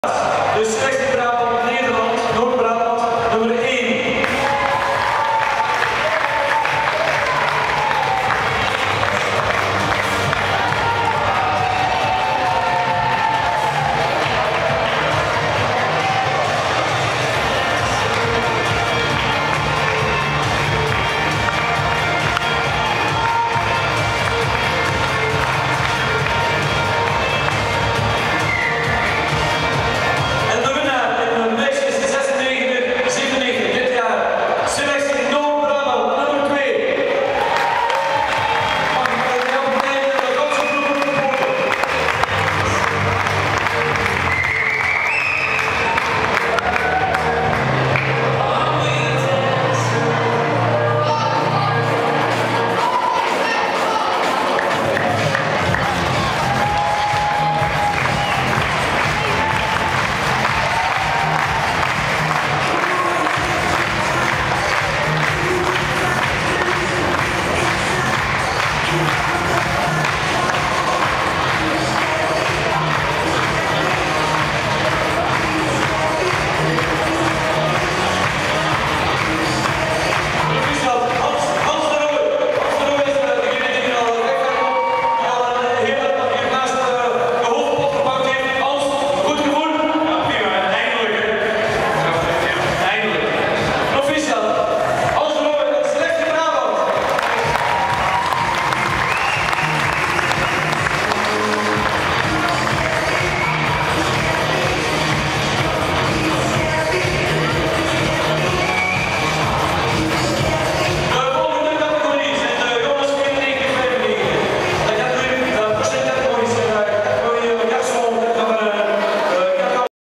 handbalselectie Brabant meisjes jong
Handbaltoernooi Ledegancktoernooi Eeklo 27/6 t/m 28/6 2009